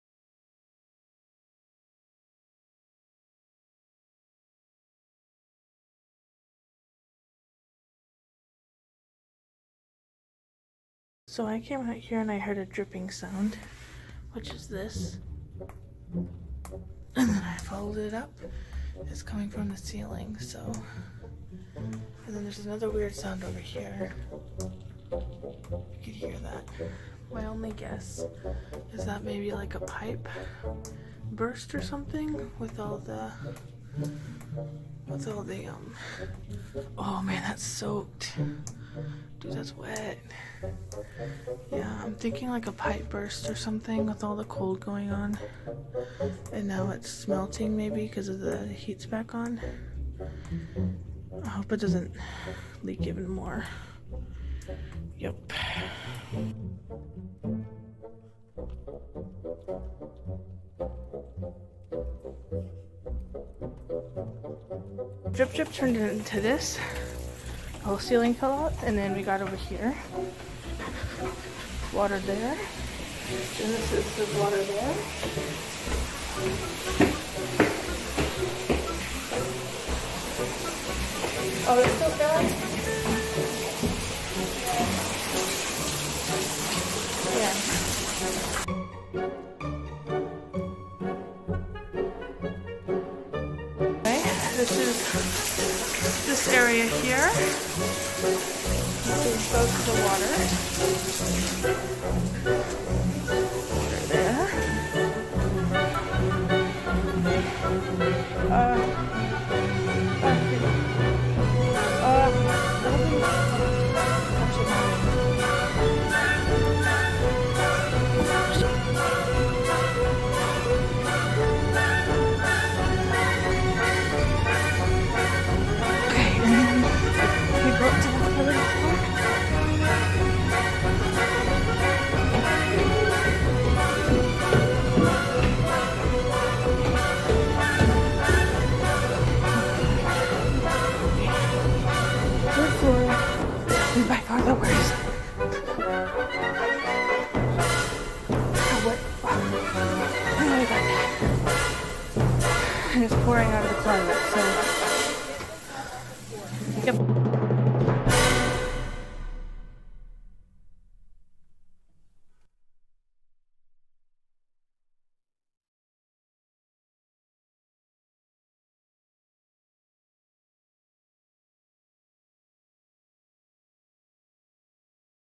Pipeburst at Dallas Baptist University girls dorm- Crowley!
The dramatized telling of the night of burst pipes at DBU. 🤟😜✌